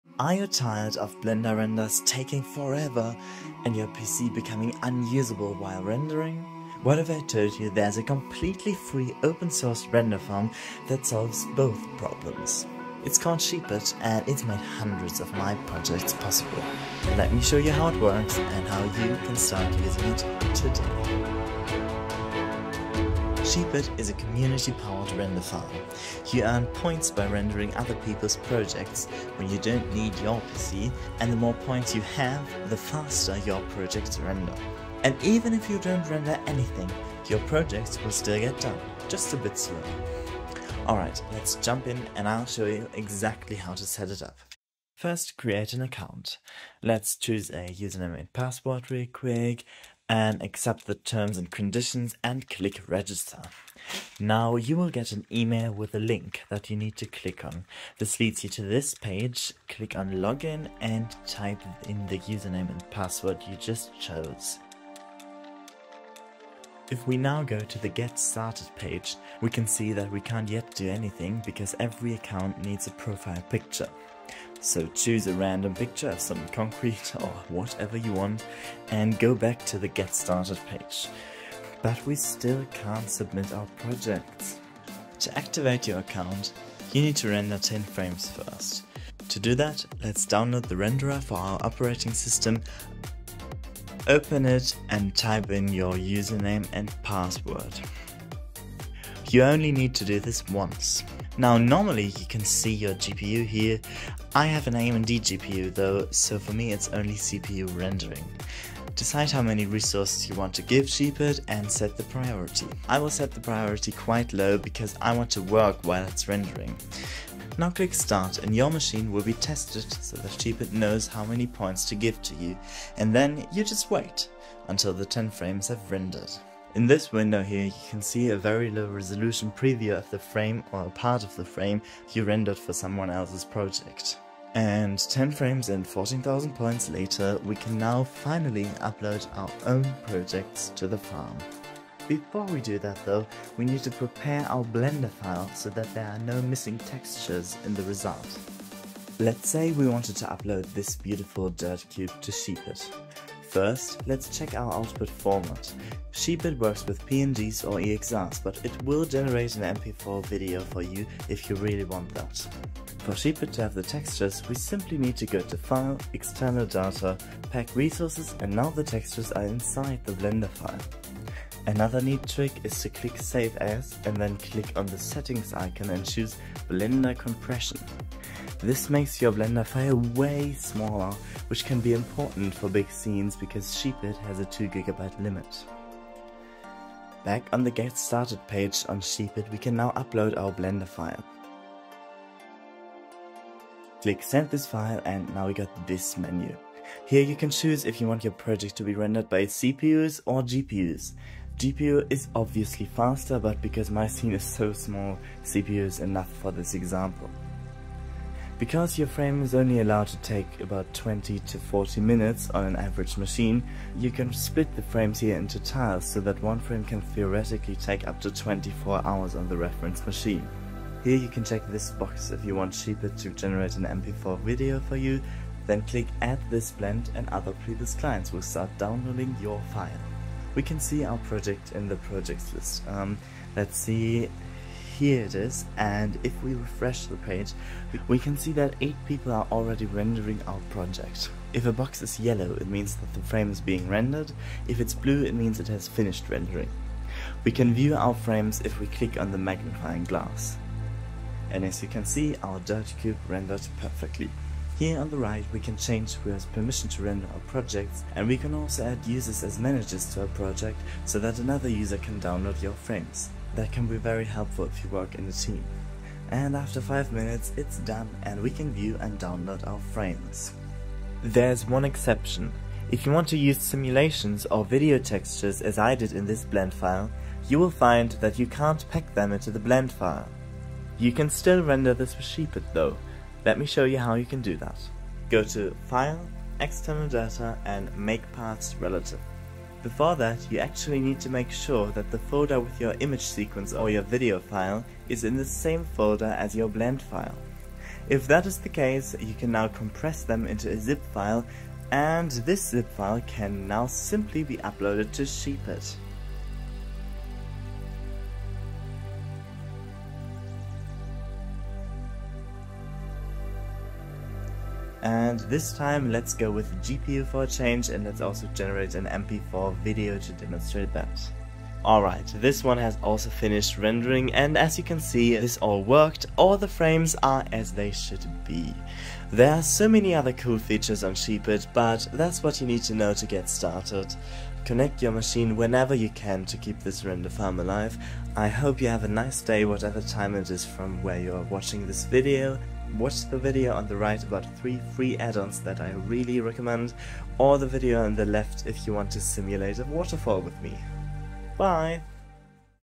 Render FAST & FREE in Blender 4.5 with SheepIt Renderfarm!
Happy Rendering!
I hope SheepIt can help you with your projects!